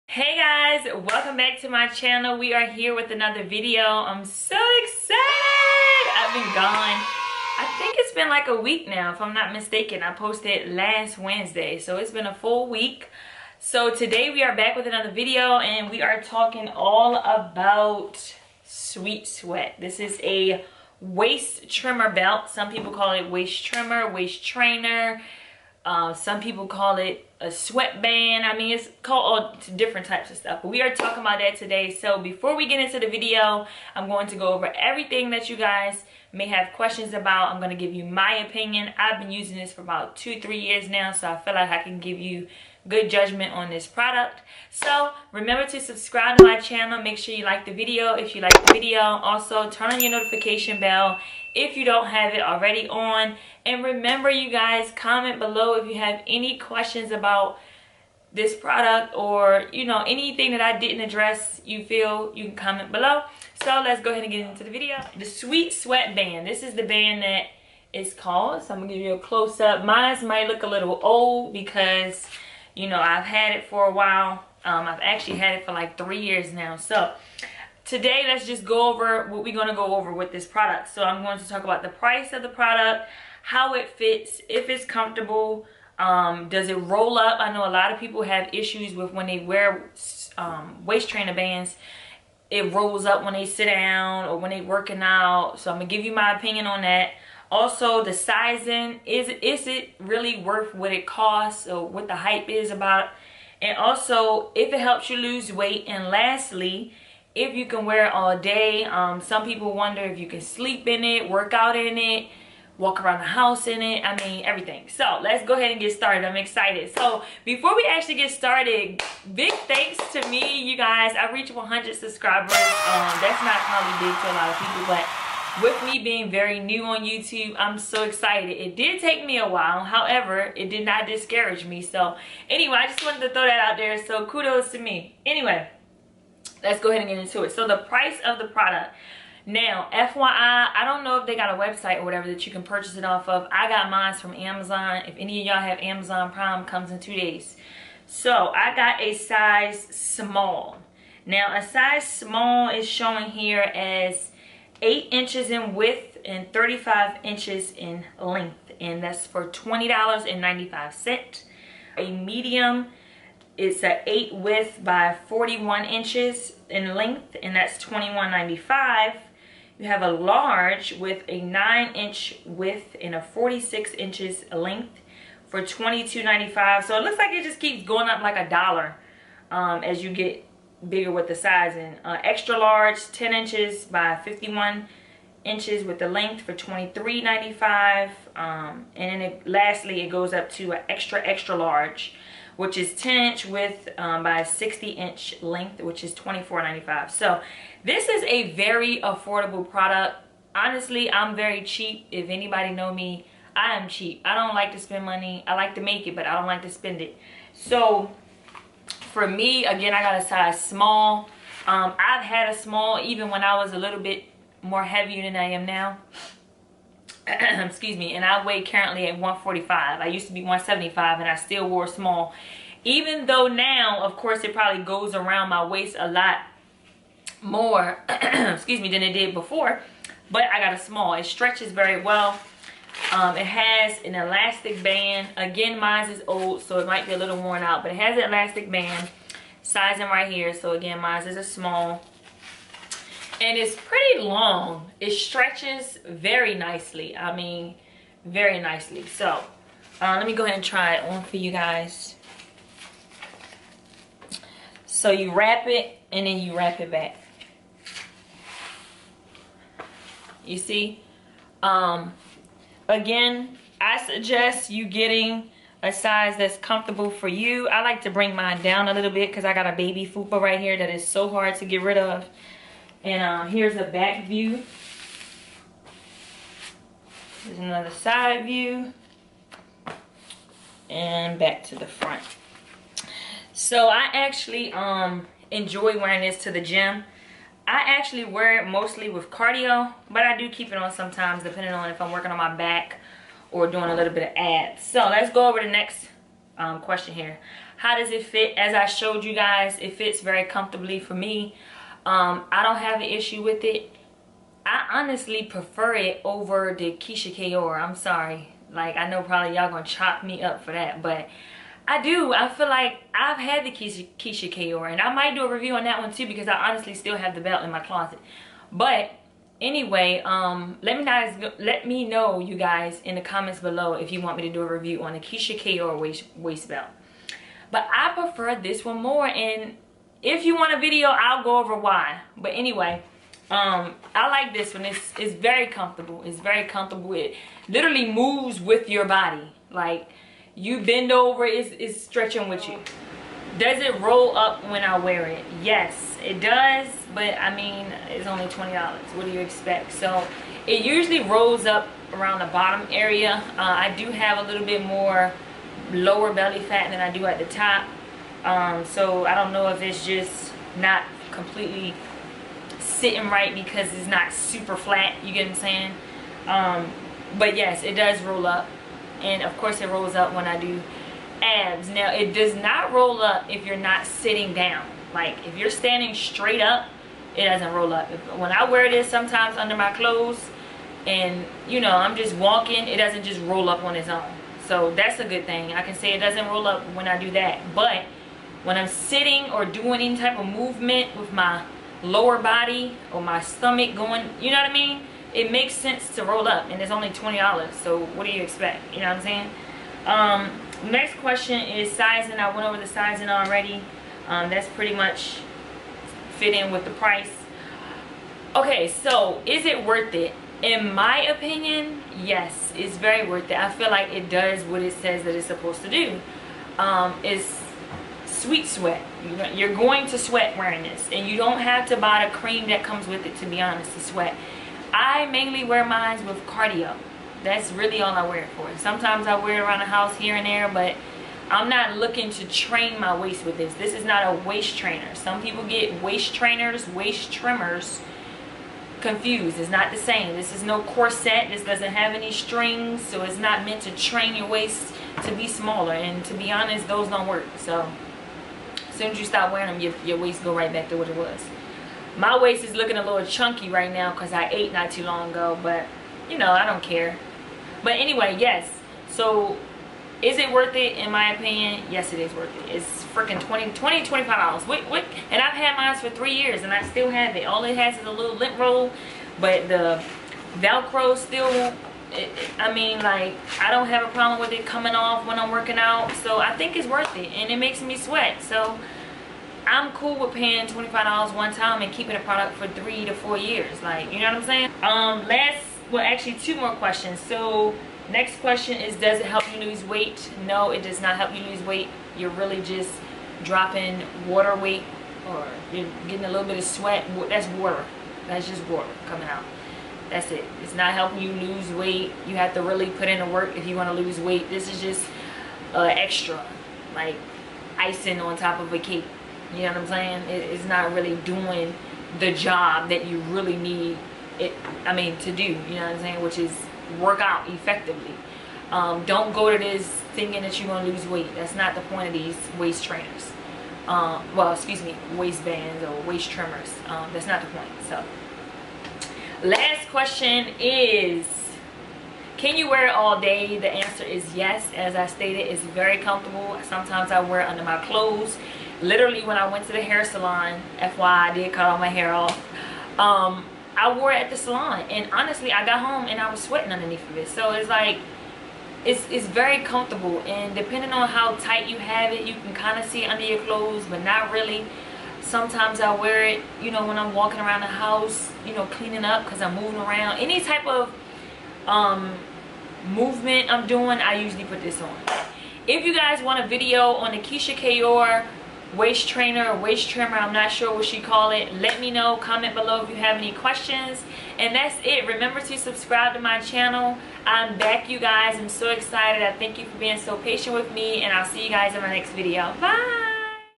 SWEET SWEAT WAIST TRIMMER REVIEW! | DOES IT REALLY WORK?? | IS IT BETTER THAN KA'OIR WAIST ERASER??
Hey guys! I filmed a video reviewing the SweetSweat waist trainer that I've had now for about 2 years. Let me know what you guys think in the comments below. Also let me know if you think it is better than the KeyshiaKa'oir waist eraser belt. Thank you shy subs! 💕

Name: Rashaya Boston
Age: 24
Ethnicity: Black/African American